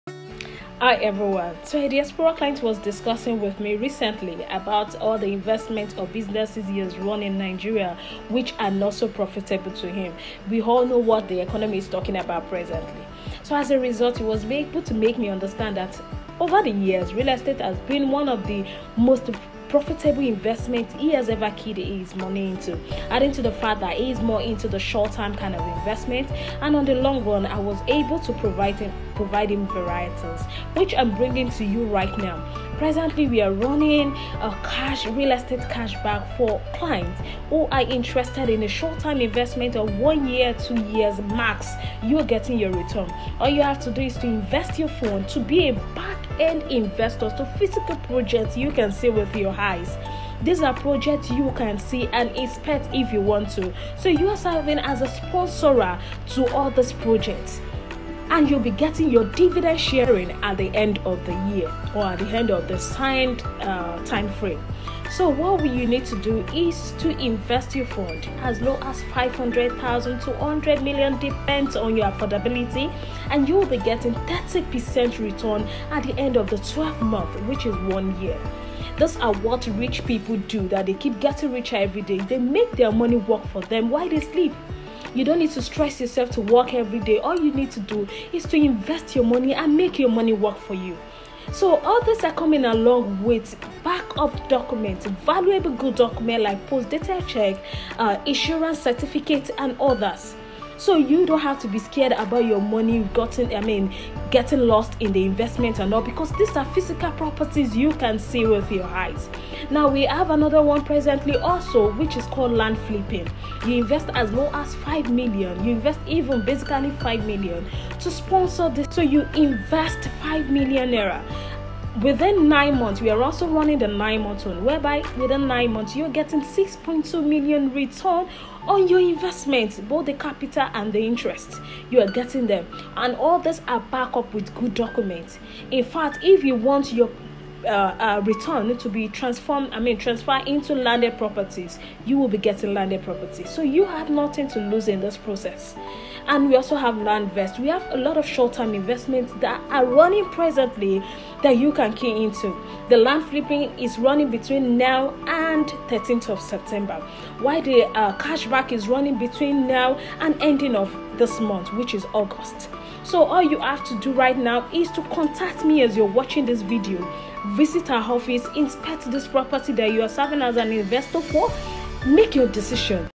Real Estate Investment: How to be a Multi Millionaire Investing in Real Estate.
The Reason why the rich are getting Richer:

1. They are informed, information is the key
2. They don’t procrastinate in making firmed decisions
3.They play risk smartly, with focus
4. They make their money work for them and not keep working for the money.
5. They make use of opportunity and never let it slip out of them.

I can continue on and on to 1000, offers like this are great means of making your money work for you, Real Estate Short term investment is the means to a continuous cashflow without stress, these are tangible investment with physical properties seen, so you be assured your money can’t go into flames.

Fixed deposit in the bank can’t give you up to even 20% in a year , it even depends on the kind of account you are running.

Real Estate Cashback/Property Flipping is a short term investment where you invest your fund to sponsor Real Estate Project and get back your capital with interest at the end of the signed time, past clients have made this valid decision and are grateful for the outcome

Document coming along with your investment is an assurance that your fund is safe or can be changed into physical property depends on your choice.

This offer last between Now and 13th of September.